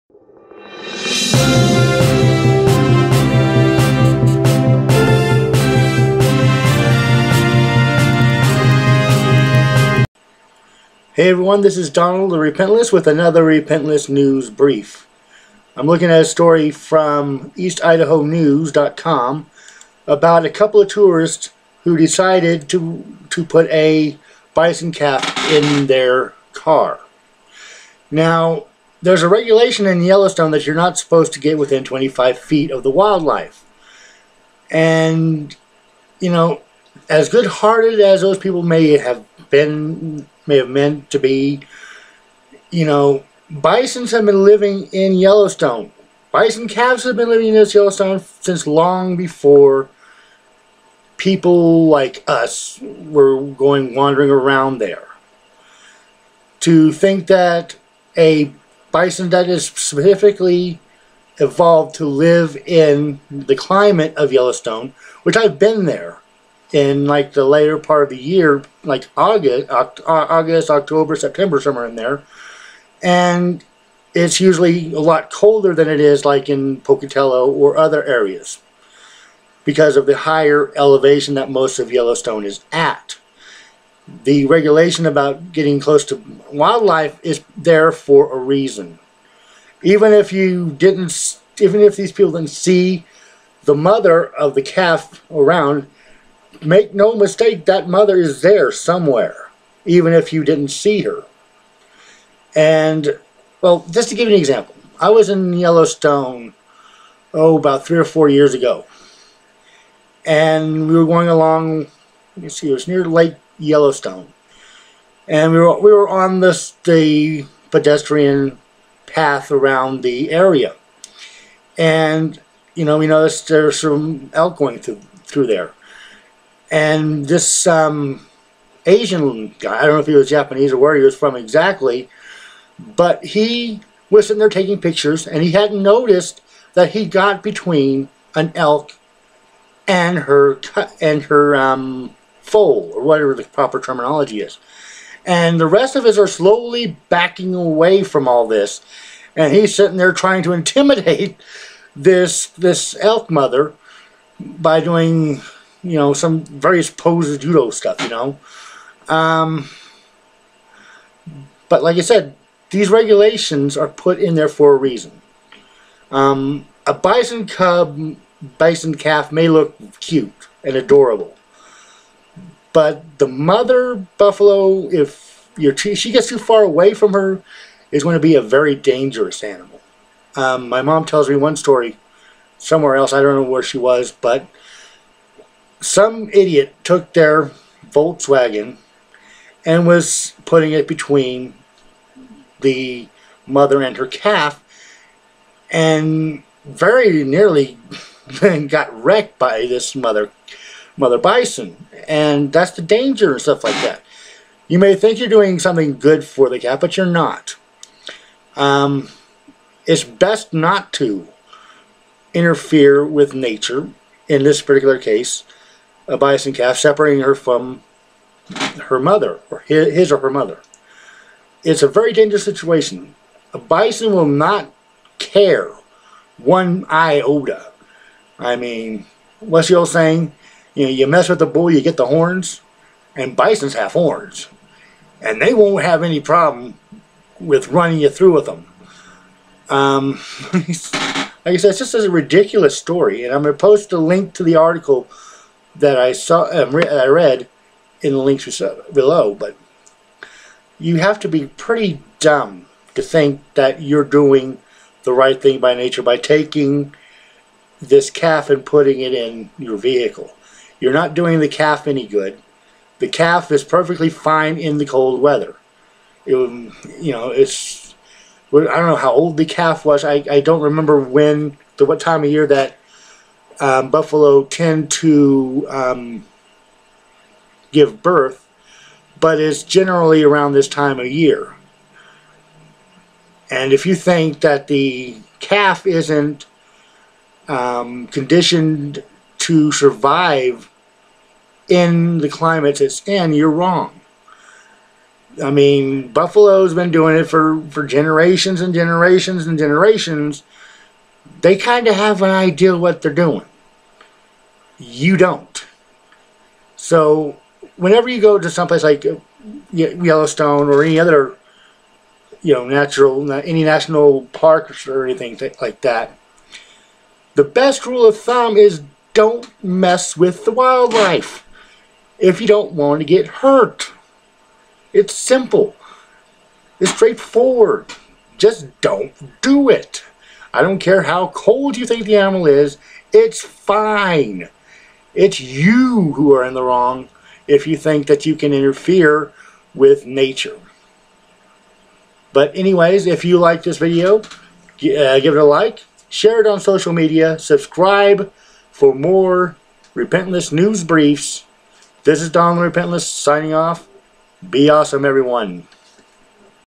Tourists in Yellowstone Put Bison Calf in Their Vehicle
A couple of tourists do the wrong thing by putting a Bison calf in their vehicle. Get tickets for "kind" act.